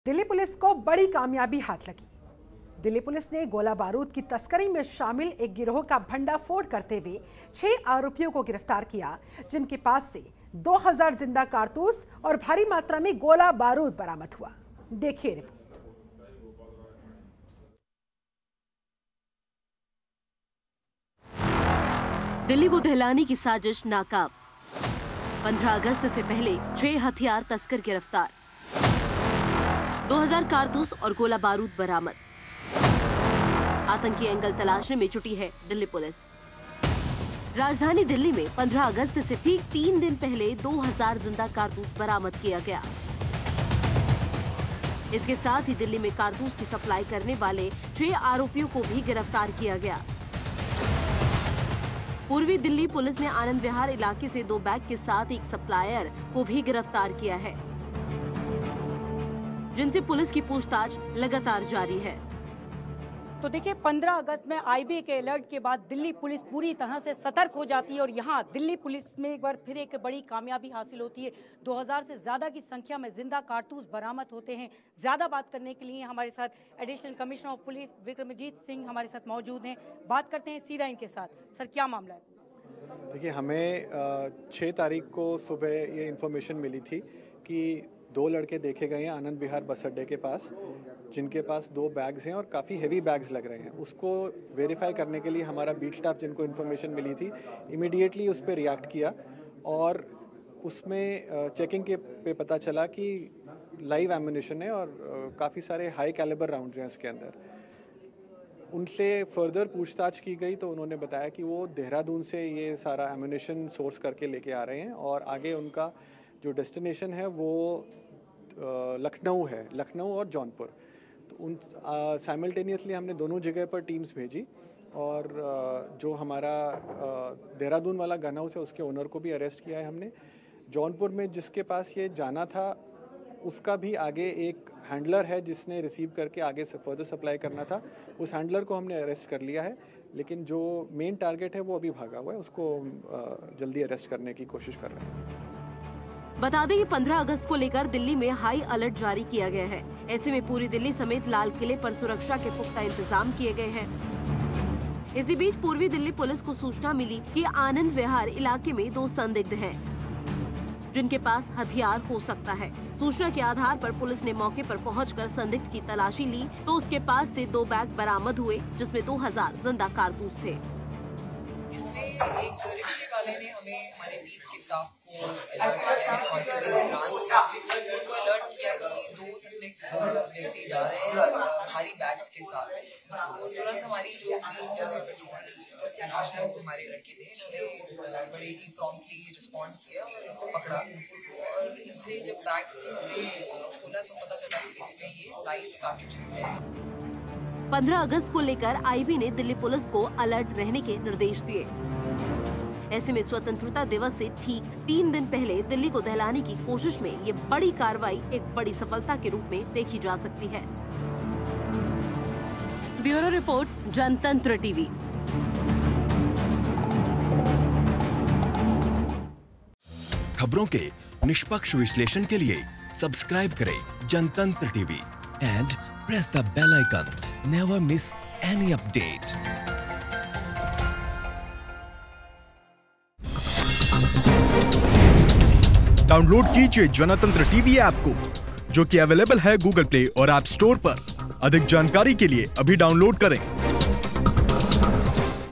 15 August से पहले छह हथियार तस्कर गिरफ्तार | 2000 कारतूस और गोला-बारूद बरामद
About Video:
दिल्ली को दहलाने की साजिश नाकाम
15 अगस्त से पहले छह हथियार तस्कर गिरफ्तार
2000 कारतूस और गोला-बारूद बरामद
आतंकी एंगल तलाशने में जुटी दिल्ली पुलिस